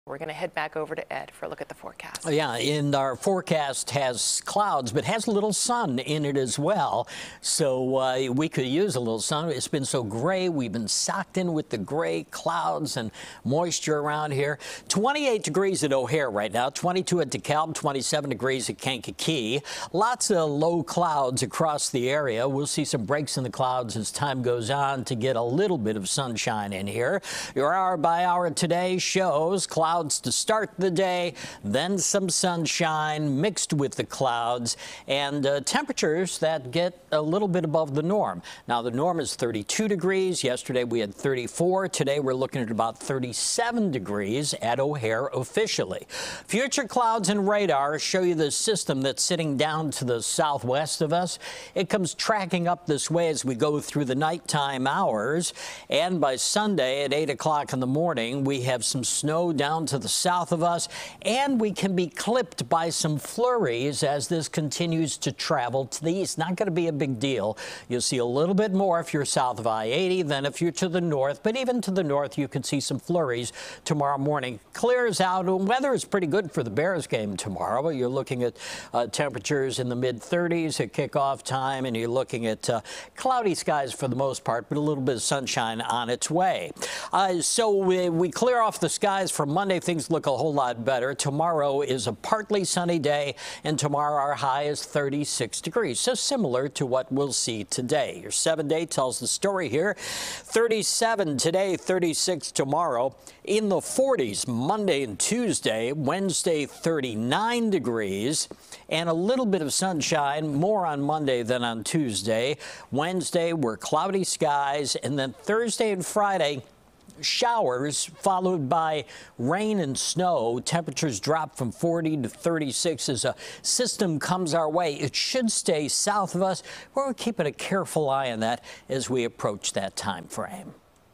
First Alert Weather: Peeks of sunshine Saturday
It'll be a quiet weather weekend with temps slightly above what's typical and some sunshine to squint at.